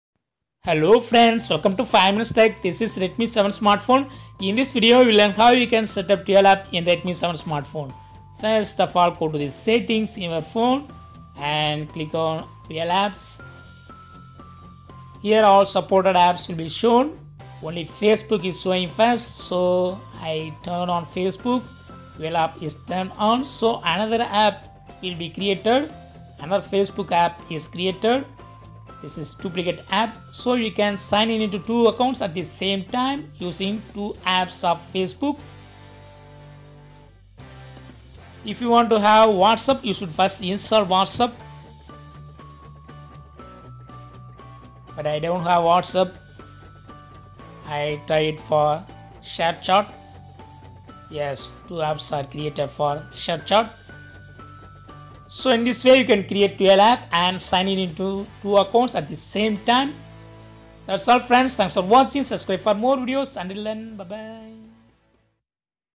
How to set up dual apps in Redmi 7
Redmi 7 dual apps settings: Just go to the settings and click on dual apps. Just click on the button against facebook,  what's app,  twitter, sharechat or any other supported apps for setting dual app. You can sign in into two accounts at the same time in your Redmi 7 smartphone.